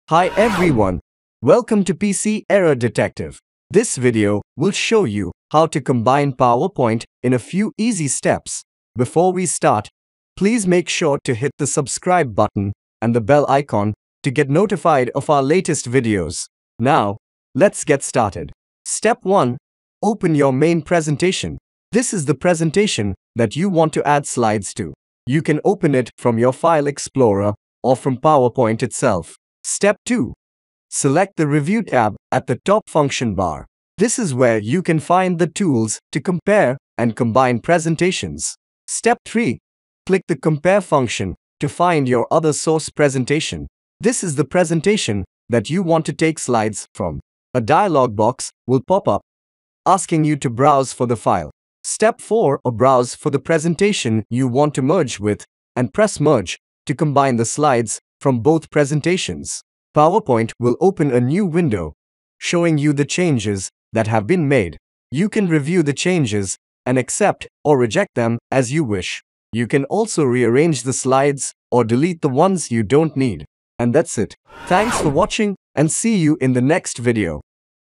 How to Combine PowerPoints | Elevate Your Slides
How to Combine Powerpoints

Unlock the full potential of your presentations by learning how to combine multiple PowerPoints into one cohesive slide deck. This tutorial will walk you through the process, ensuring a smooth transition between presentations. Ideal for professionals and students alike, you'll discover how to enhance your storytelling by merging ideas and visuals effectively. Get ready to captivate your audience with a polished, unified presentation.

- Merging PowerPoint presentations
- Combining slides in PowerPoint
- PowerPoint presentation tips
- Effective presentation techniques
- Unified slide creation